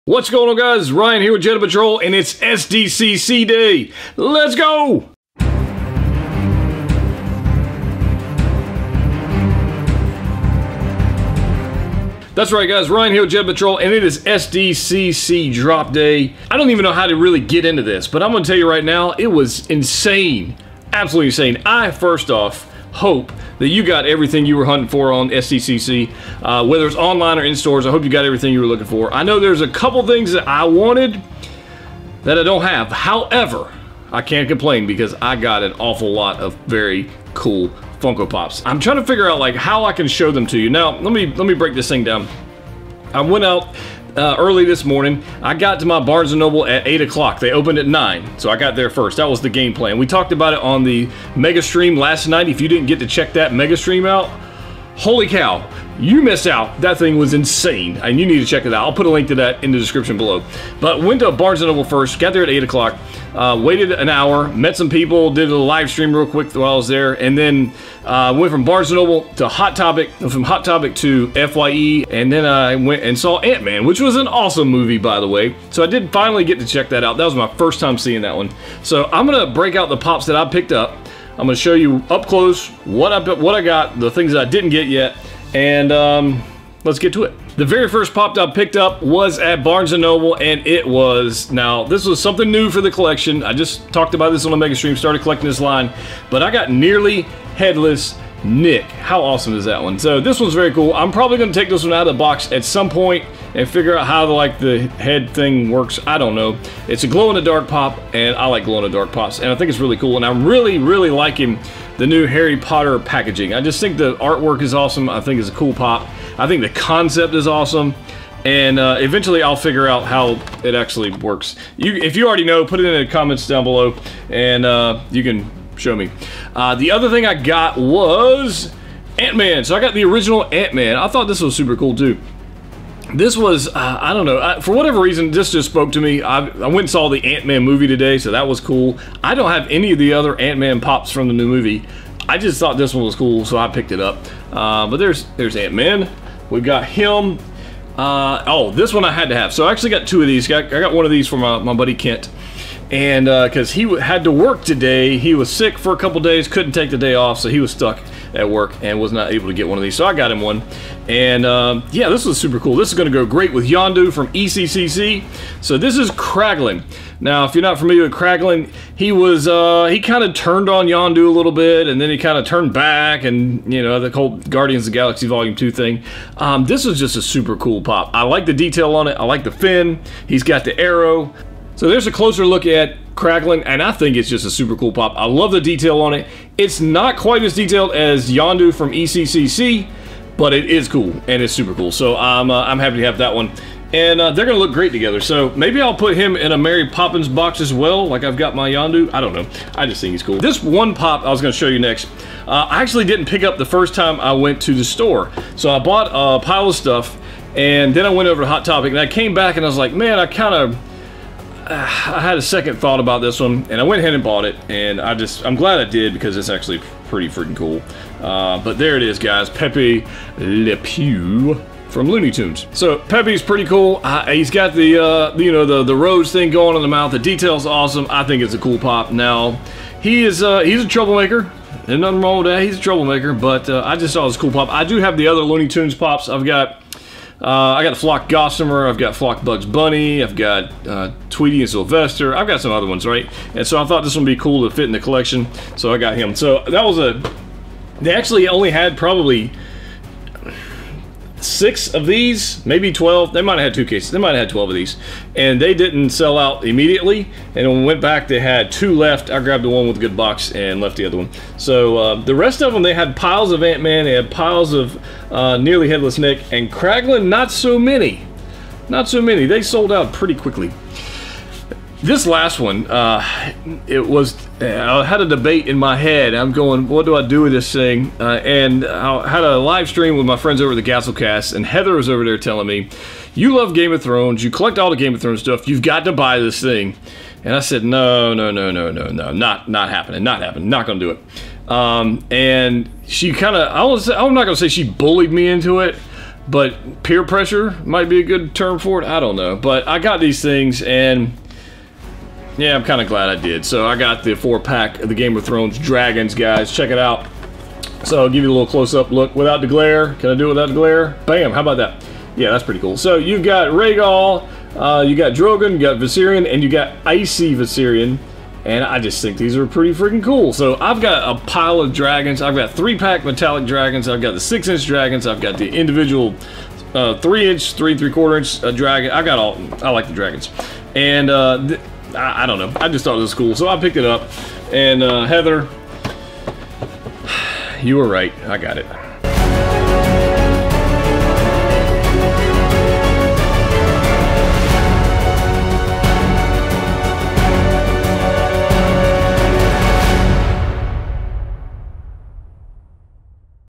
My 2018 SDCC Funko Pop Haul - Part 1 - Barnes and Noble
Check out Part 1 of my Epic SDCC Funko Pop Haul. There were just too many to share in 1 video. Keep an eye out for the next video coming very soon!

Live Streams each Sunday Night at 9pm

Send Letters to:
Jedha Patrol

Mail Packages To